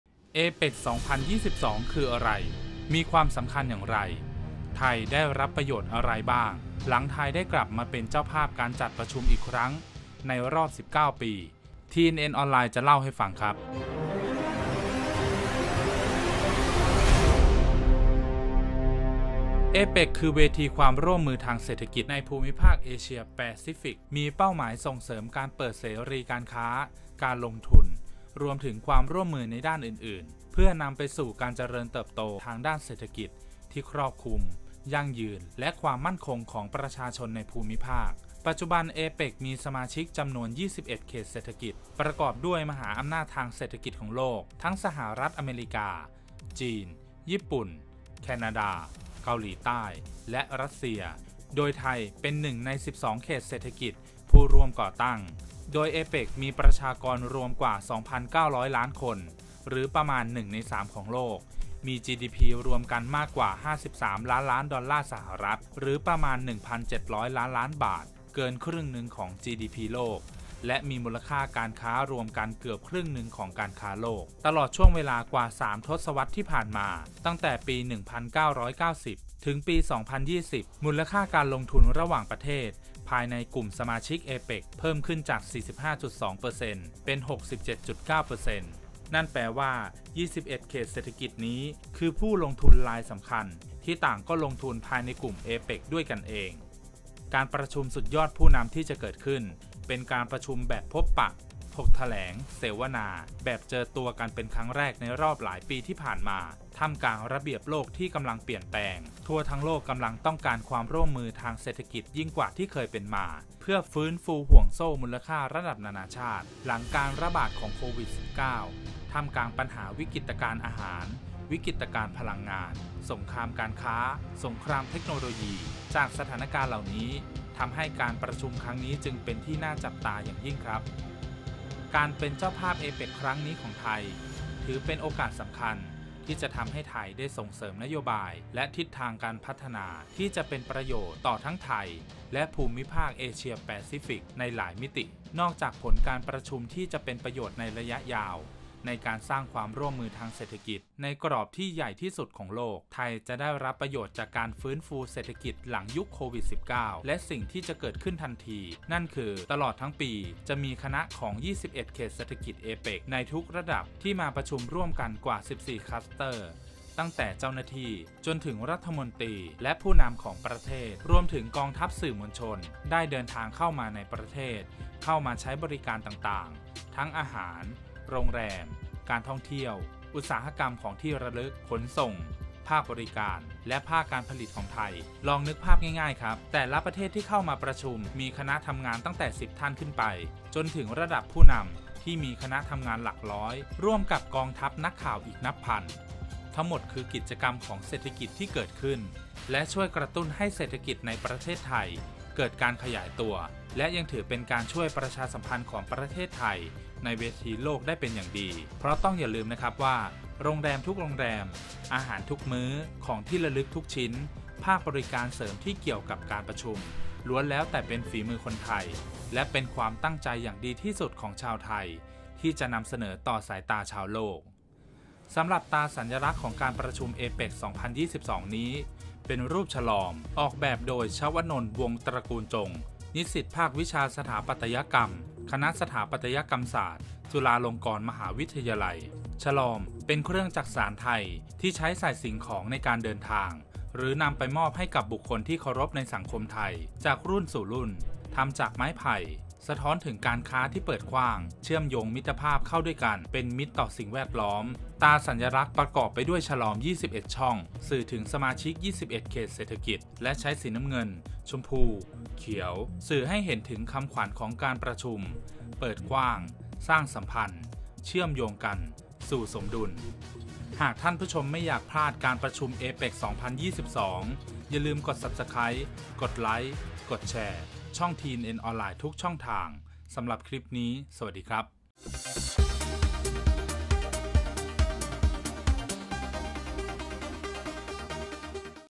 APEC 2022 เอเปค คืออะไร? ไทยได้ประโยชน์อะไรบ้าง ทำไม “ชะลอม” จึงเป็นสัญลักษณ์เอเปค 2022
APEC 2022 เอเปค คืออะไร? มีความสำคัญอย่างไร ไทยเป็นเจ้าภาพได้ประโยชน์อะไรบ้าง แล้วทำไม “ชะลอม” จึงมากลายเป็นสัญลักษณ์เอเปค 2022
เอเปค คือเวทีความร่วมมือทางเศรษฐกิจในภูมิภาคเอเชีย-แปซิฟิก มีเป้าหมายส่งเสริมการเปิดเสรีการค้า-การลงทุน รวมถึงความร่วมมือในด้านอื่นๆ เพื่อนำไปสู่การเจริญเติบโตทางเศรษฐกิจที่ครอบคลุม ยั่งยืน และความมั่งคงของประชาชนในภูมิภาค
เอเปคสมาชิกจำนวน 21 เขตเศรษฐกิจ มีประชากรรวมกว่า 2,900 ล้านคน หรือประมาณ 1 ใน 3 ของโลก  มี (GDP) รวมกันกว่า 53 ล้านล้านดอลลาร์สหรัฐ หรือประมาณ 1,700 ล้านล้านบาท เกินครึ่งของ GDP โลกและมีมูลค่าการค้ารวมกันเกือบครึ่งหนึ่งของการค้าโลก ซึ่งทำให้มูลค่าการลงทุนเติบโตระหว่างประเทศภายในกลุ่มสมาชิก APEC อย่างต่อเนื่อง
ไทยได้ประโยชน์อย่างมากในการเป็นเจ้าภาพเอเปคครั้งนี้ นอกจากประโยชน์ภาพรวมของประเทศ สิ่งที่เกิดขึ้นทันที โดยไม่ต้องรอการประชุมสุดยอดผู้นำ นั่นคือ ตลอดทั้งปี มีคณะของ 21 เขตเศรษฐกิจ APEC ในทุกระดับ ที่มาประชุมกันรวมแล้วกว่า 14 คลัสเตอร์ ตั้งแต่ เจ้าหน้าที่ จนถึงรัฐมนตรี และผู้นำของประเทศ รวมทั้งกองทัพสื่อ ได้เดินทางเข้ามาในประเทศ ทำให้เกิดการเงินเข้าประเทศตลอดทั้งปี และยังเป็นการประชาสัมพันธ์สถานที่ต่างๆ ของประเทศไทยไปด้วย
ส่วน “ชะลอม” มาเป็นสัญลักษณ์งานประชุมระดับโลกนี้ได้อย่างไร ต้องรับชมในคลิปครับ